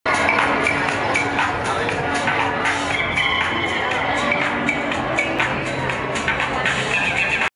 3D art in Creators Project tent @ Coachella 2011
Some 3D art in the Creators Project tent @ Coachella 2011 in Indio, CA on 04/15/11.